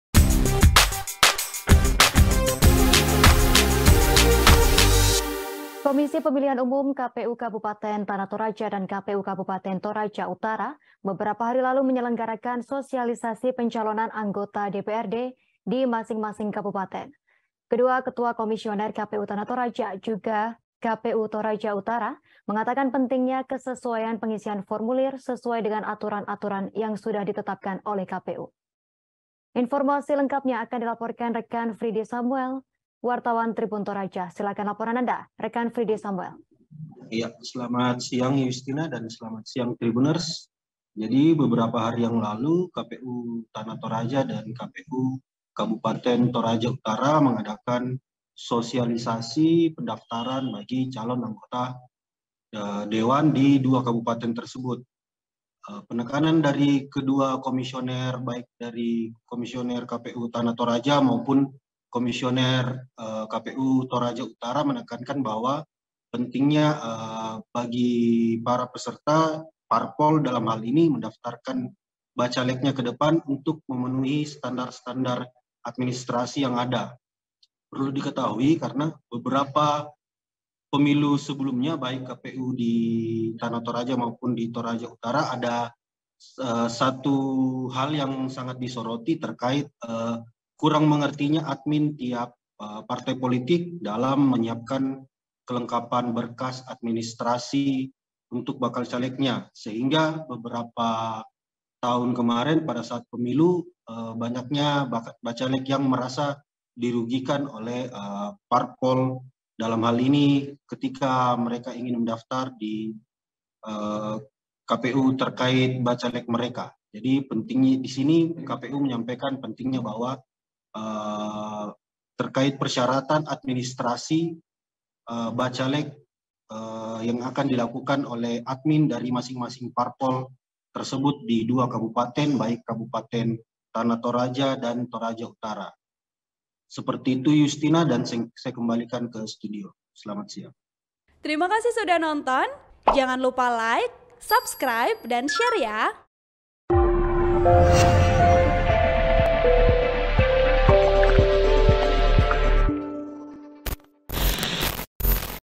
Parpol Kerap Lalai Persyaratan Administrasi, KPU di Toraja Sosialisasikan Kelengkapan Berkas Bacaleg
Komisi Pemilihan Umum (KPU) Kabupaten Tana Toraja, dan KPU Kabupaten Toraja Utara, beberapa hari lalu menyelenggarakan Sosialisasi Pencalonan Anggota DPRD di masing - masing kabupaten.

Kedua Ketua Komisioner KPU Tana Toraja, juga KPU Toraja Utara, mengatakan pentingnya kesesuaian pengisian formulir sesuai dengan aturan - aturan yang sudah ditetapkan oleh KPU.